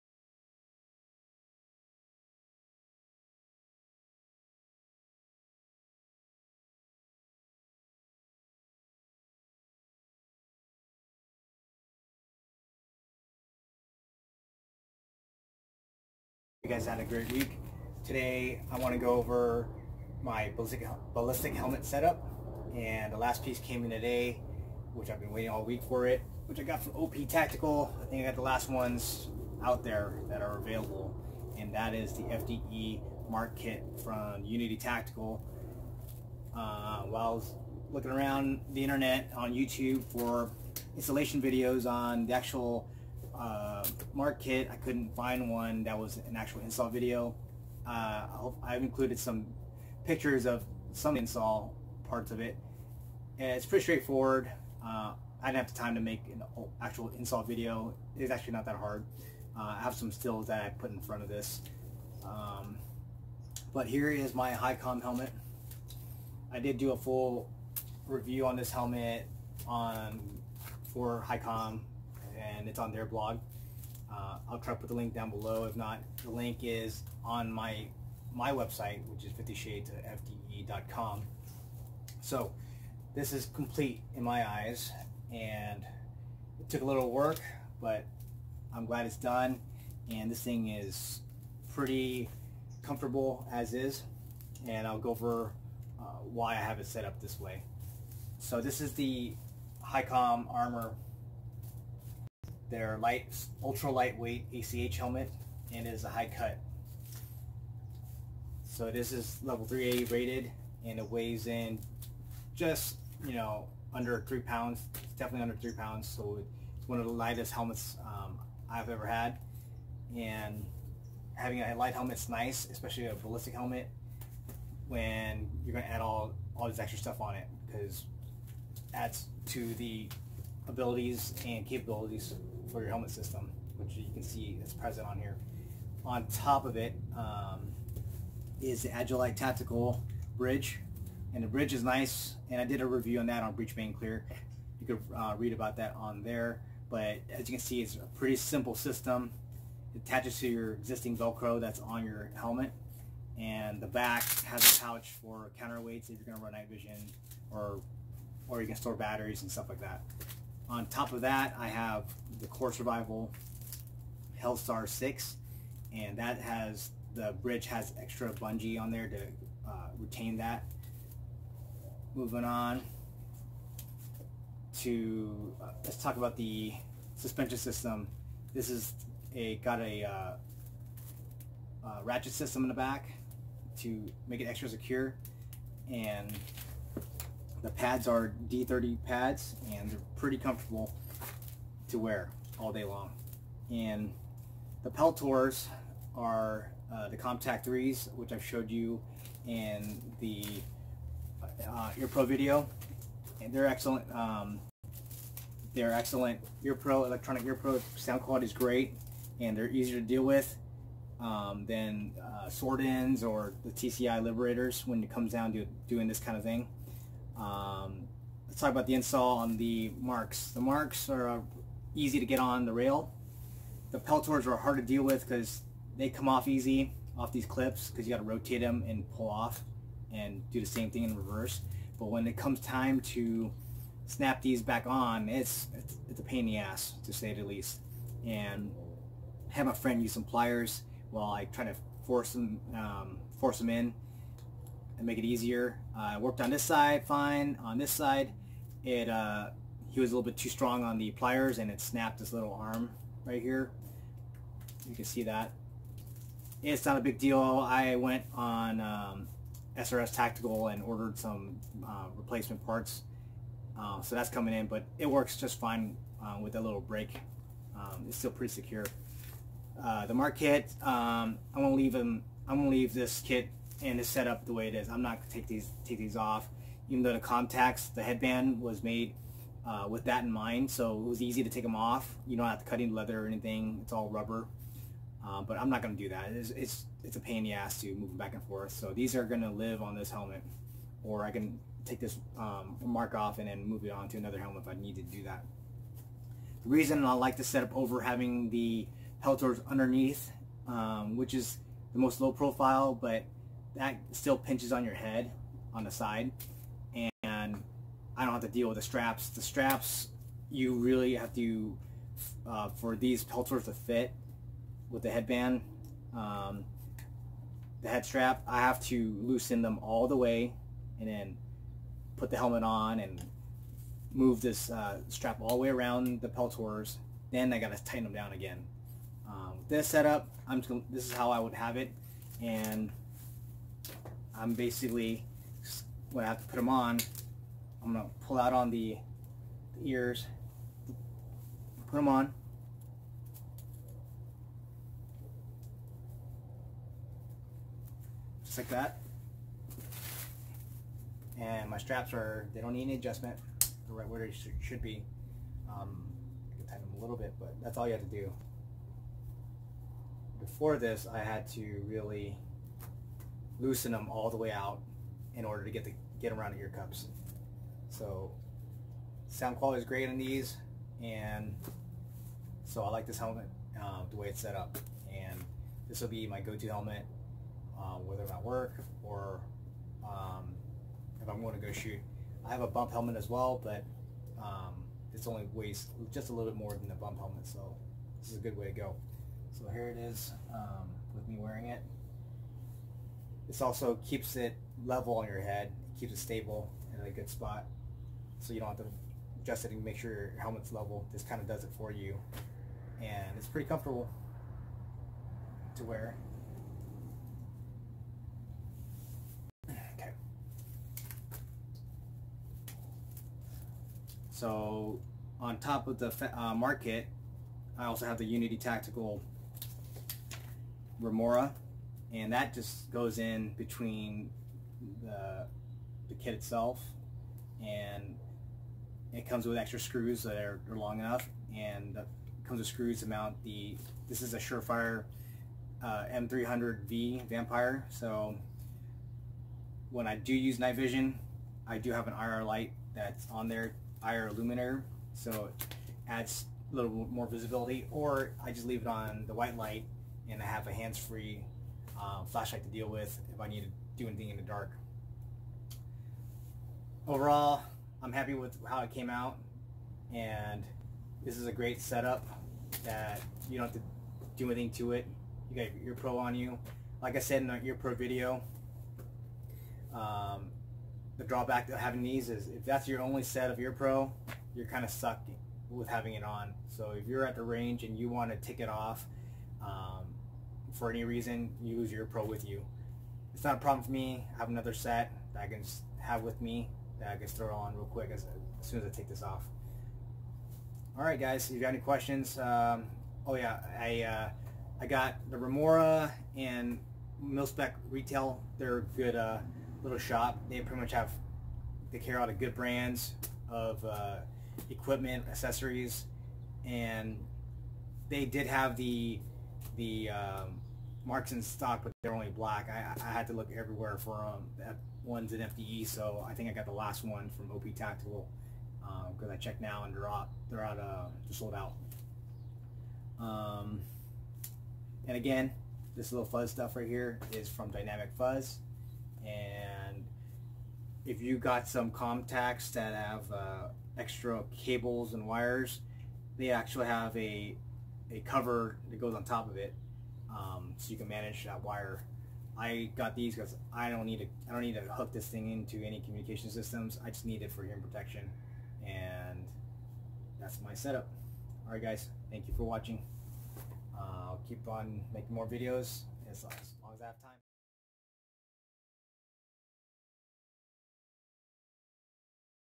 Highcom Armor Ballistic Helmet setup with Unity Tactical MARK / Remora. Peltor Comtac IIIs
My Ballistic Helmet Setup and why.
Highcom Armor Ultra Lightweight ACH Review:
Agilite Tactical Bridge
Unity Tactical MARK / Remora
Peltor Comtac III
CORE Survival Helstar 6
Dynamicfuzz velcro kit